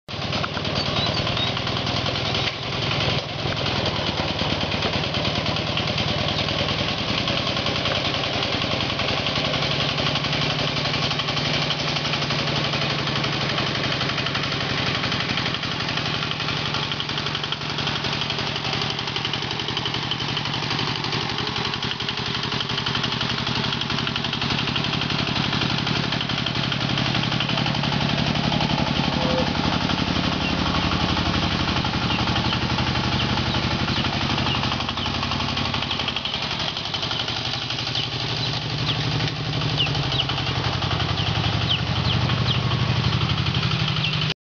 Минитрактор с мотоблока ЗАРЯ + перевозка сена
Выполним модернизацию любого мотоблока в многофункциональный минитрактор.